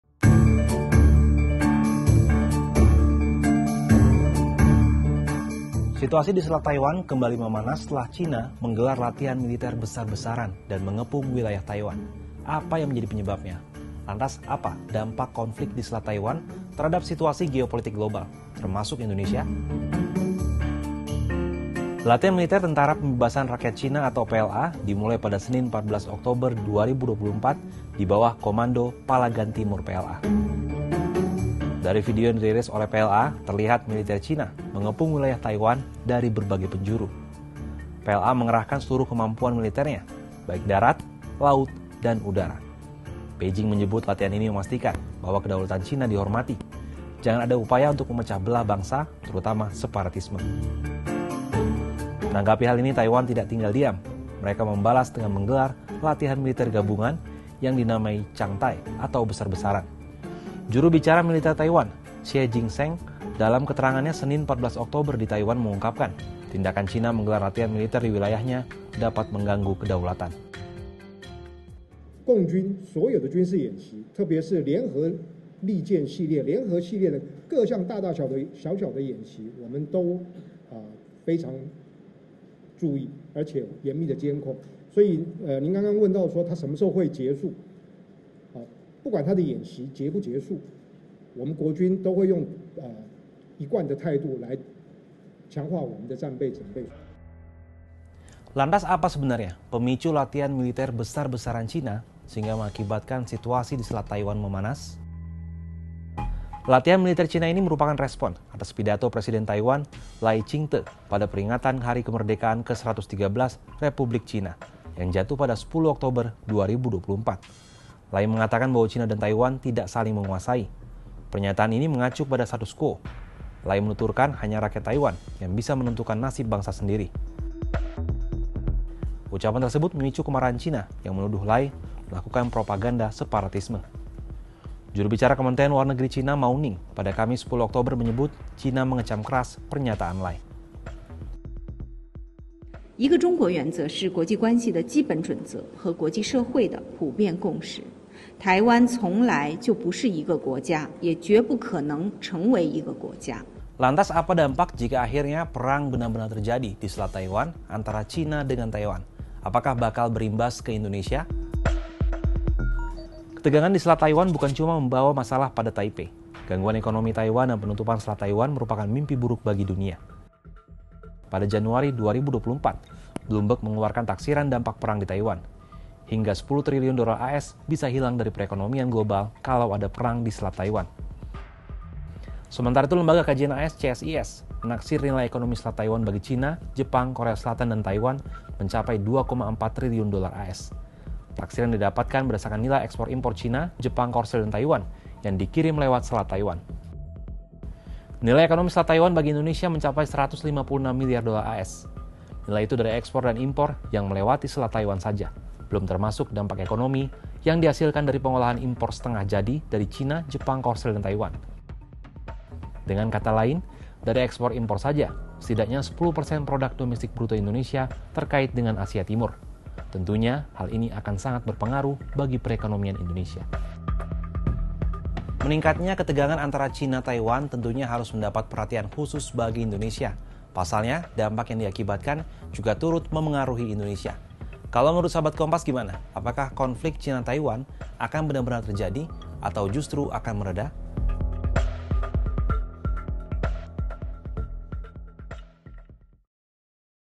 China-Taiwan Memanas, Apa Dampaknya Bagi Indonesia?
Pada Senin (14/10/2024) China menggelar latihan militer bersandi Pedang Gabungan 2024B (Joint Sword-2024B). Latihan militer Tentara Pembebasan Rakyat China (PLA) ini berlangsung di bawah Komando Palagan Timur PLA. Dalam latihan tersebut, PLA mengepung seluruh perairan Taiwan di sembilan titik. Tujuannya, mencegah kedatangan bantuan asing dan menghalangi jalur perdagangan internasional.